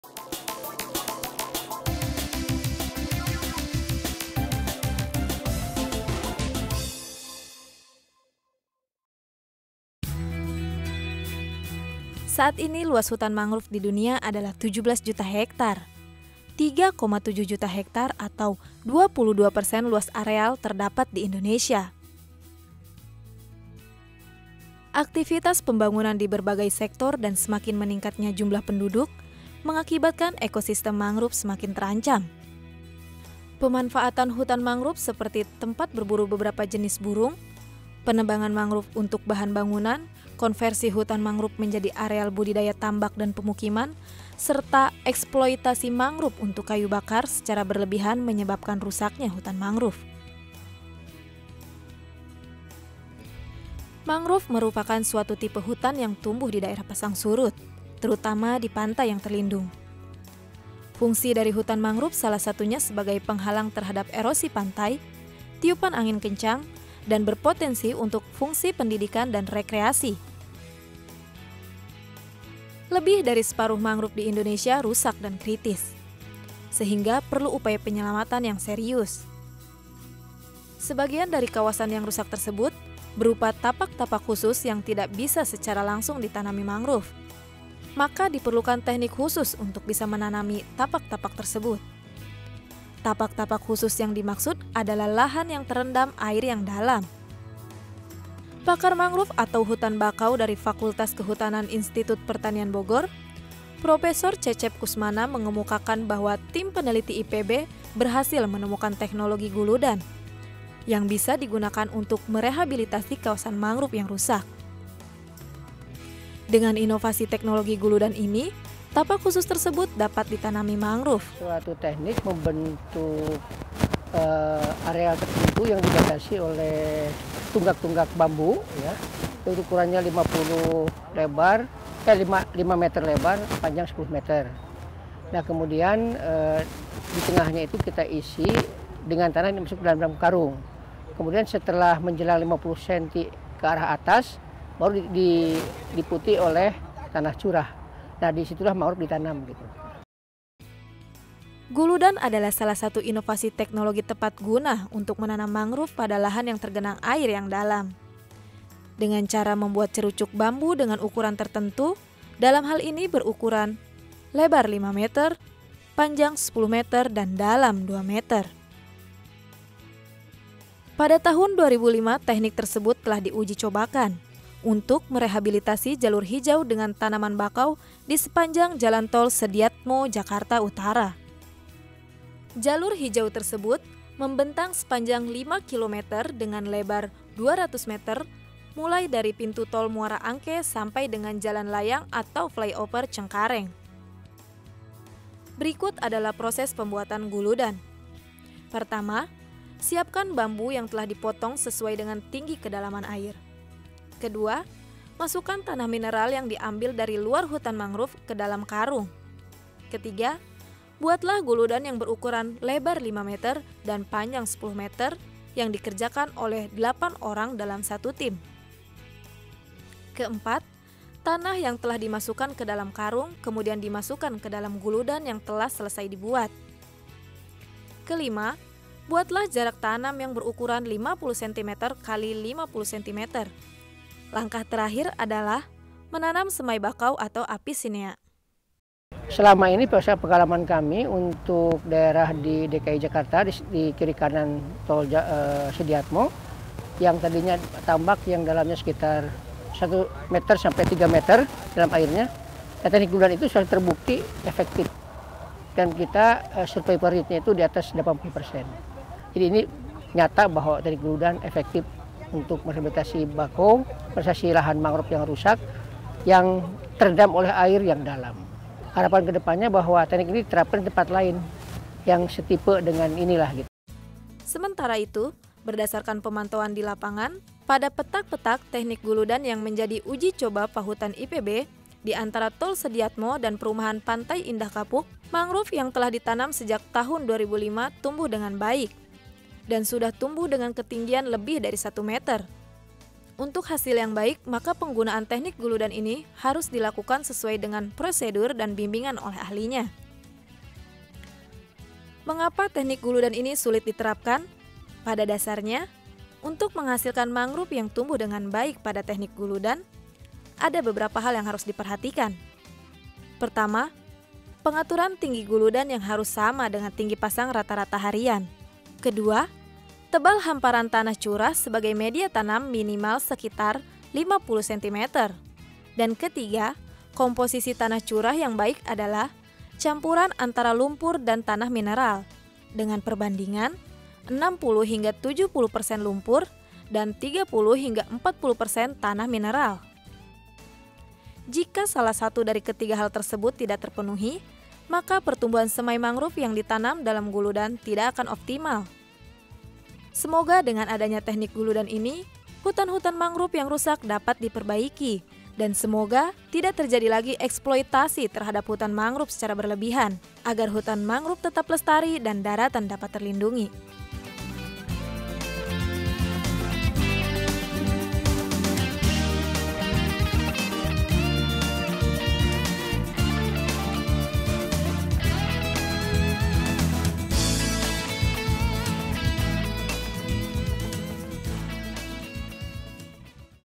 Teknik Guludan untuk Penanaman Mangrove
Mangrove merupakan suatu tipe hutan yang tumbuh di daerah pasang surut, terutama di pantai yang terlindung. Lebih dari 50% kawasan mangrove di Indonesia berada dalam kondisi rusak. Salah satu teknik yang bisa digunakan untuk merehabilitasi kawasan mangrove yang rusak adalah dengan teknik guludan. Teknik guludan ini adalah salah satu inovasi teknologi tepat guna untuk menanam mangrove pada lahan yang tergenang air yang dalam, dengan cara membuat "guludan" cerucuk bambu dengan ukuran tertentu.